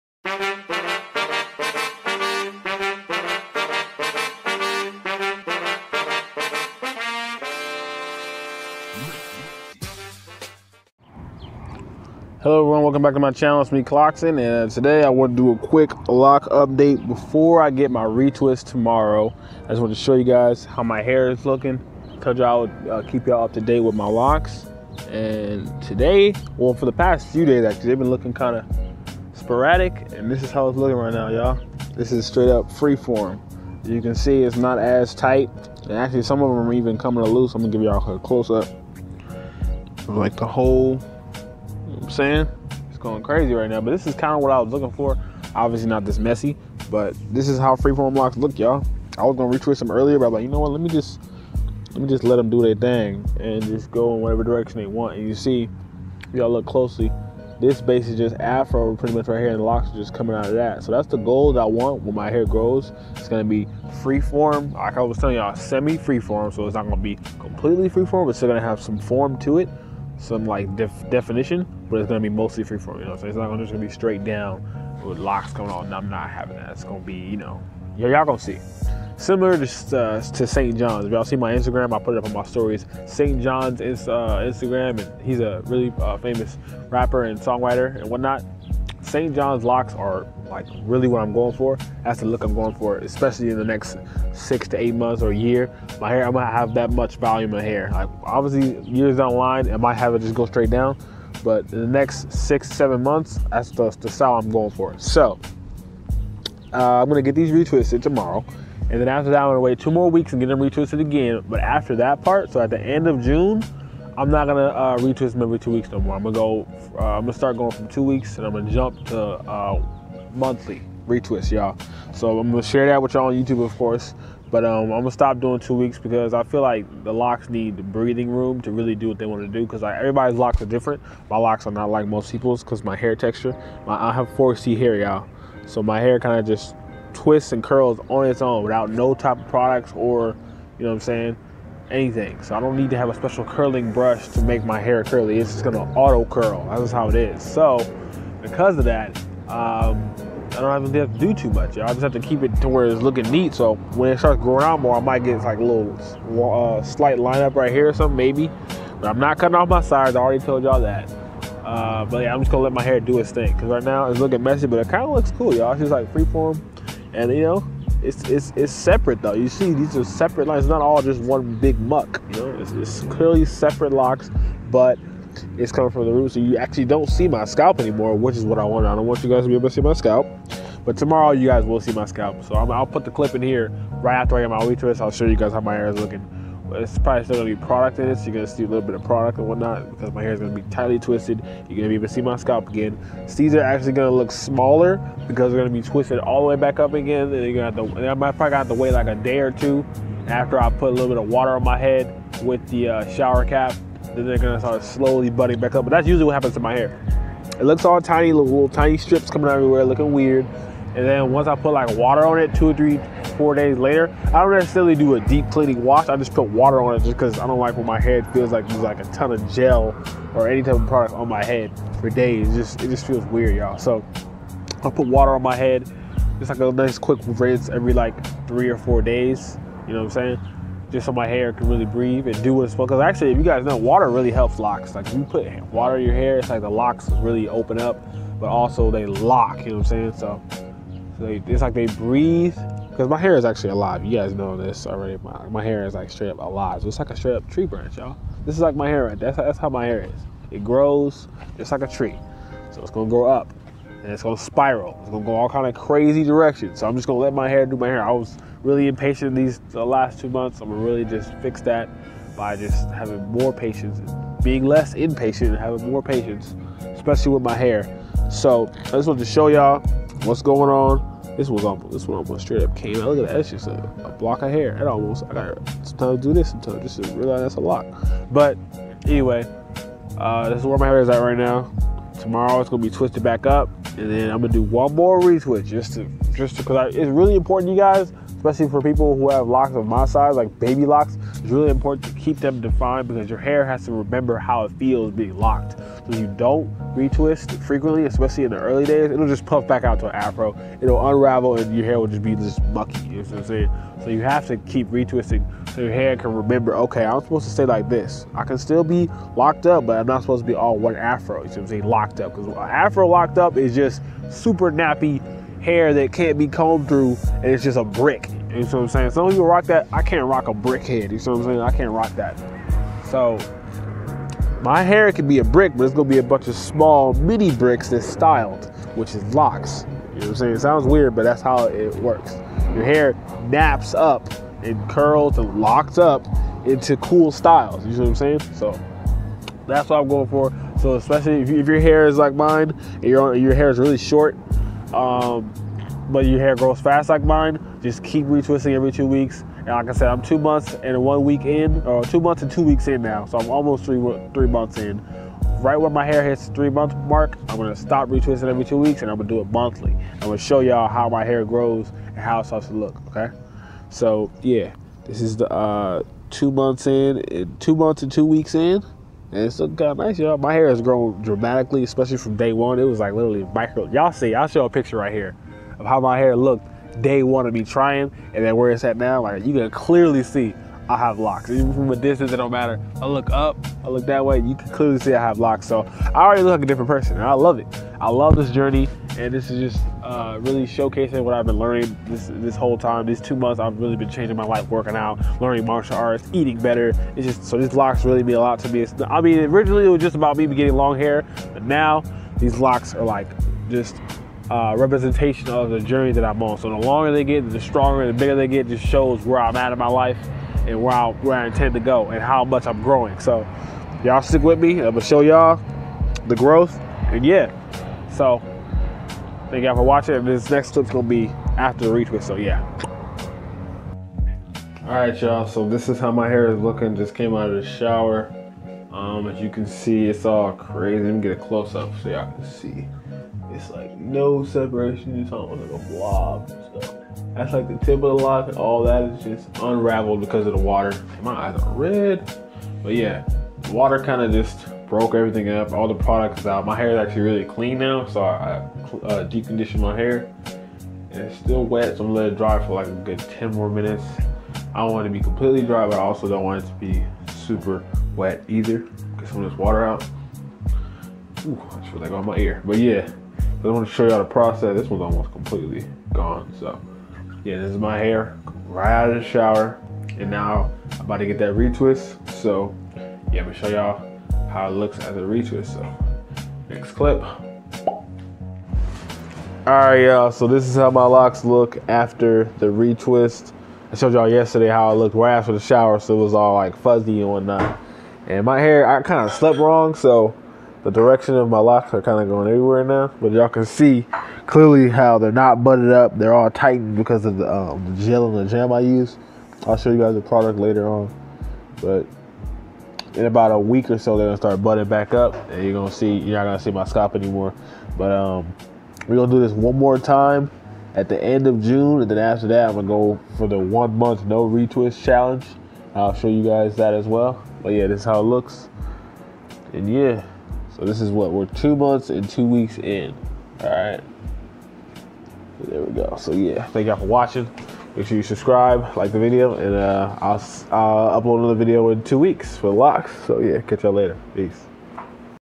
My freeform locs are showing 2 mouths 2 weeks locked Retwist time (loc journey)
my loc journey 5th retwist

and maybe we can actually make a difference together

follow me i guess